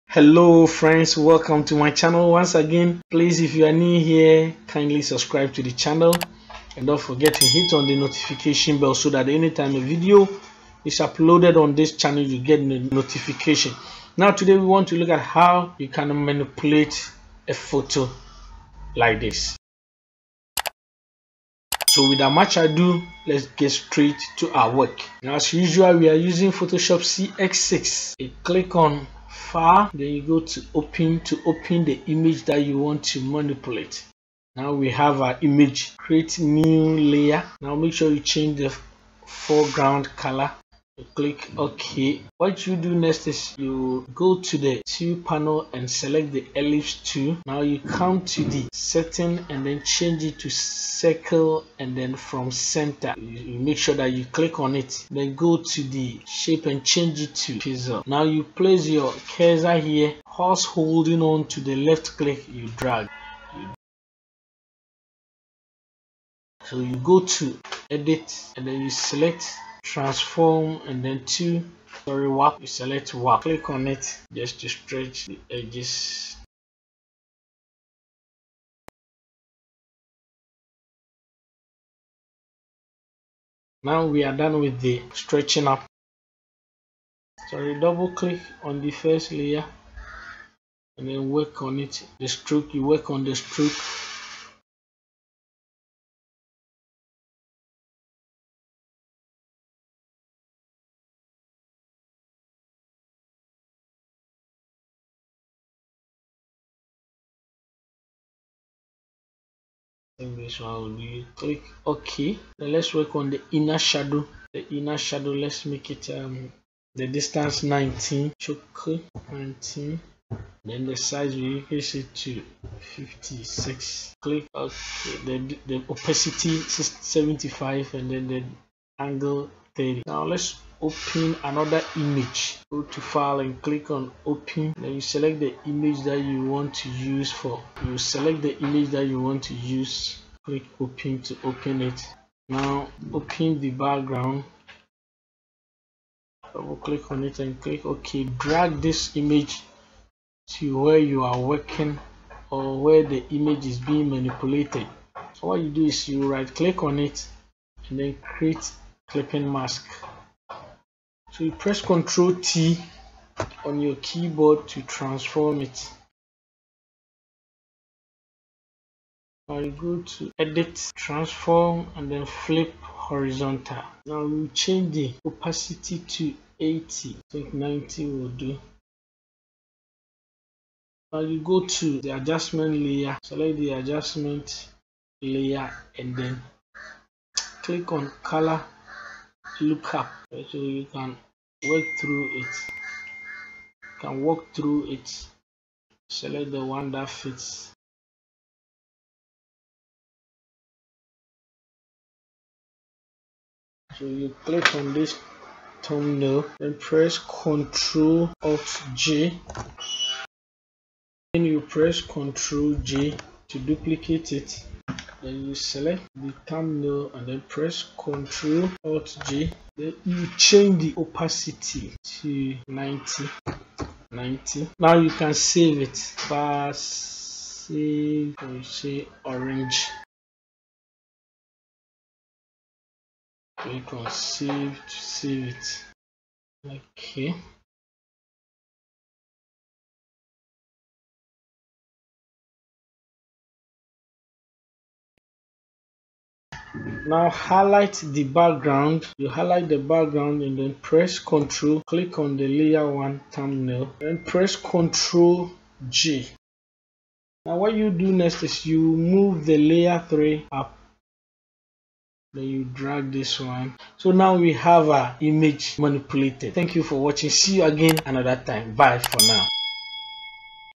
Image Manipulation { Orange and photo of a city } in Photoshop
In this Photoshop tutorial, I take you through simple 4:30 image manipulation.

Introduction - 0:13
Display          - 0:08
Tutorial         -  4:02
Conclusion   - 0:07

It is easy you can try your hands on it
Images used:
Yellow Orange
Beautiful city

All the images uses in this tutorial is solely for academic exercise